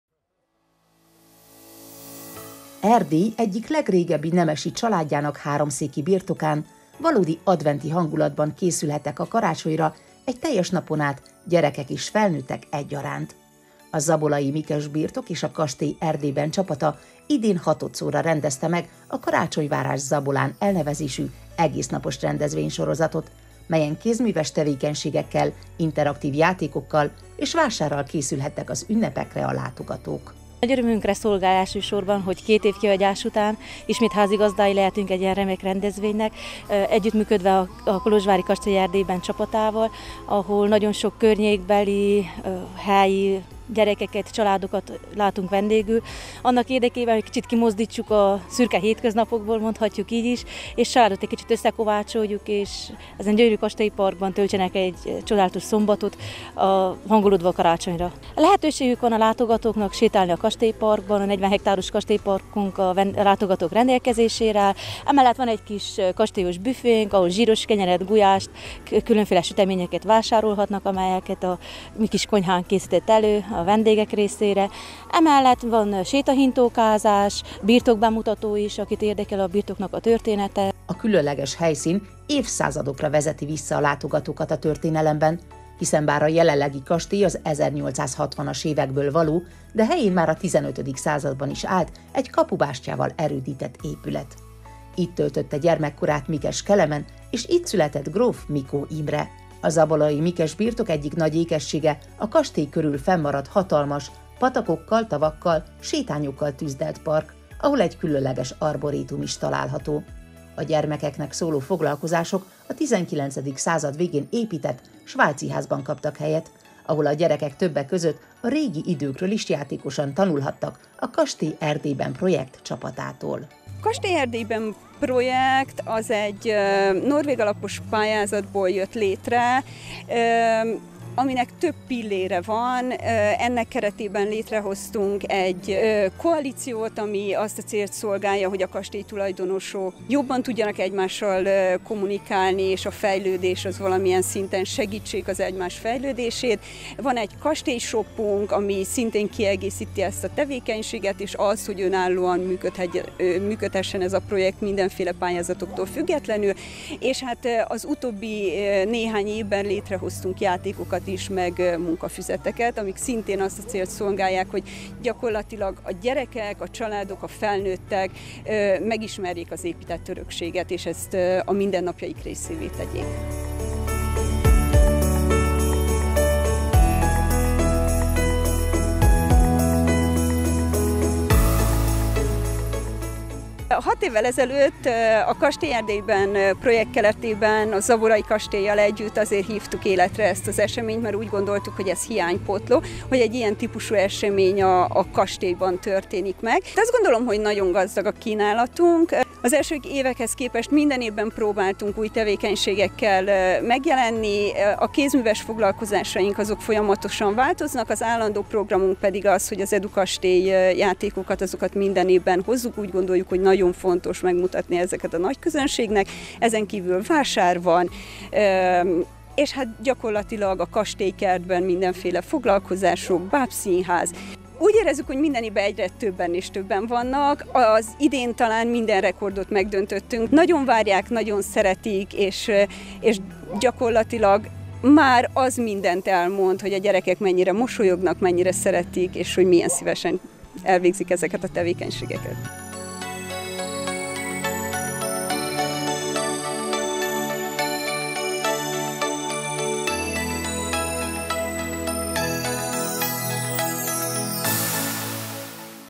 Karácsonyvárás a zabolai Mikes-kastélyban – Mozaik
Gyerekzsivalytól volt hangos a zabolai Mikes-kastély: a birtok a kolozsvári Pont csoport és a Kastély Erdélyben csapatával közösen idén hatodszorra rendezte meg egész napos ünnepváró eseményét, a karácsonyvárás Zabolán elnevezésű rendezvényt.